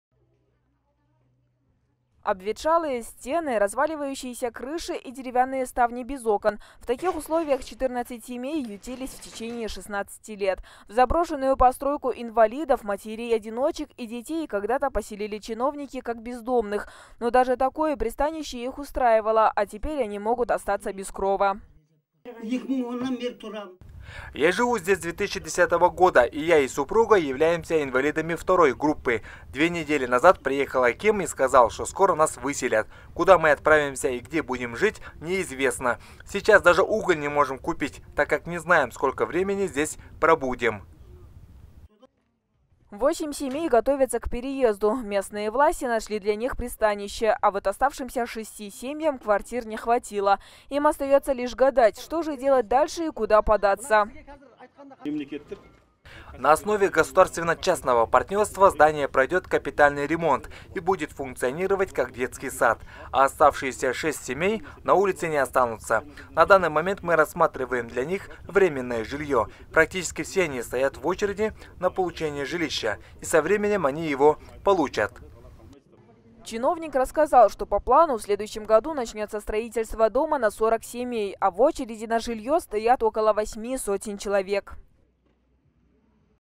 14 семей ютились в течение 16 лет в заброшенном здании.TVK 14.11.16
В Южном Казахстане из заброшенного детского сада выселяют инвалидов и матерей одиночек. У опустелого участка со старым зданием появились хозяева. А потому в преддверии зимы 6 семей в городе Шардара рискуют остаться на улице.
Обветшалые стены, разваливающиеся крыши и деревянные ставни без окон - в таких условиях 14 семей ютились в течение 16 лет. В заброшенную постройку инвалидов, матерей одиночек и детей когда-то поселили чиновники как бездомных. Но даже такое пристанище их устраивало. А теперь они могут остаться без крова.
Малик БЕКТАЕВ
«Я живу здесь с 2010 года. И я и супруга являемся инвалидами второй группы. Две недели назад приехал аким и сказал, что скоро нас выселят. Куда мы отправимся и где будем жить не известно. Сейчас даже уголь не можем купить, так как не знаем, сколько времени здесь пробудем».
Восемь семей готовятся к переезду. Местные власти нашли для них пристанище. А вот оставшимся шести семьям квартир не хватило. Им остается лишь гадать, что же делать дальше и куда податься.
Богенбай СУЙЕТОВ - заместитель акима г. Шардары
«На основе государственно-частного партнерства здание пройдет капитальный ремонт и будет функционировать как детский сад. А оставшиеся шесть семей на улице не останутся. На данный момент мы рассматриваем для них временное жилье. Практически все они стоят в очереди на получение жилища и со временем они его получат».
Чиновник рассказал, что по плану в следующем году начнется строительство дома на 40 семей. А в очереди на жилье стоят около восьми сотен человек.